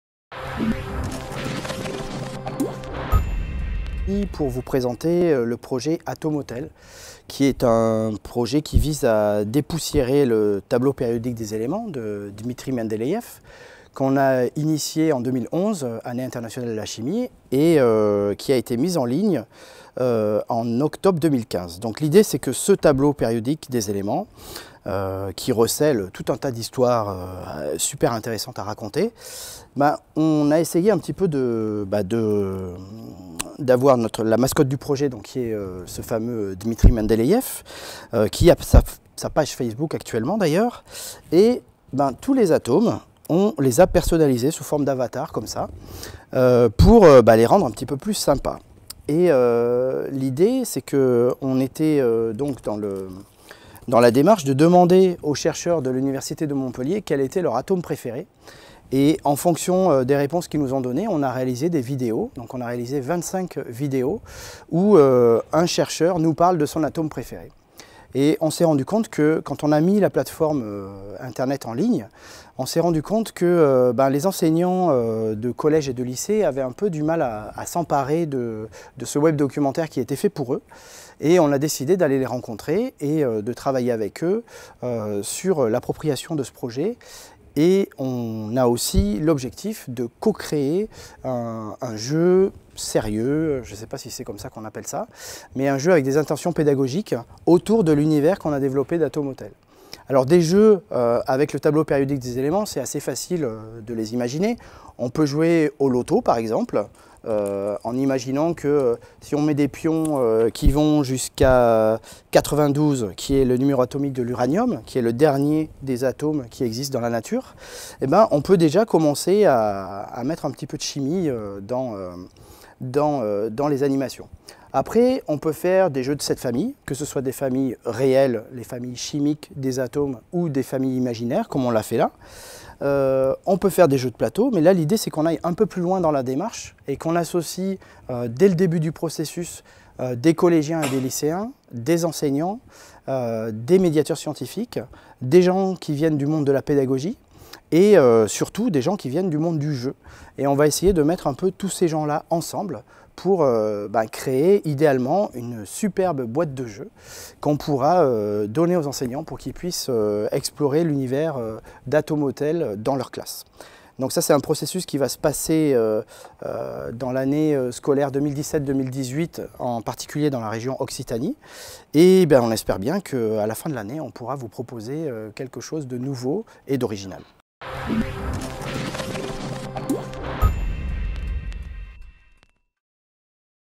Thierry Brassac (Université Montpellier) Atome Hôtel
Thierry Brassac (Université Montpellier) Atome Hôtel, un web-documentaire qui dépoussière le tableau périodique des éléments ; projet de co-création d’un jeu sérieux à partir d’un « cabinet de curiosités atomiques »

Chimie et Jeux, Sciences et Jeux : Apports dans l’enseignement  - 17 Mai 2017
+ d'info